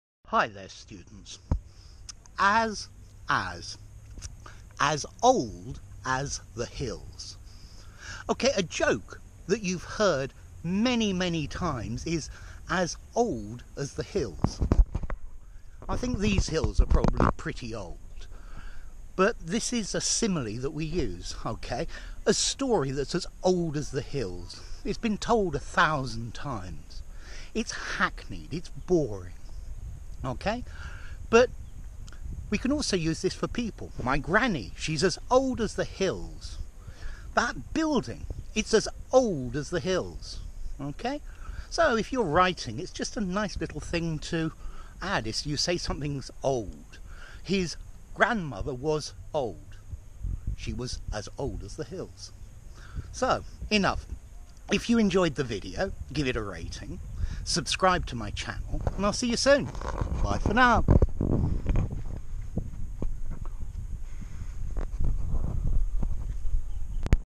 🔵 As As Collocations As Old As the Hills - Similes - As ... As - ESL British English Pronunciation
An explanation of the as as simile, as old as the hills. We use the phrase as old as the hills to mean something is ancient or hackneyed.
That story is as old as the hills.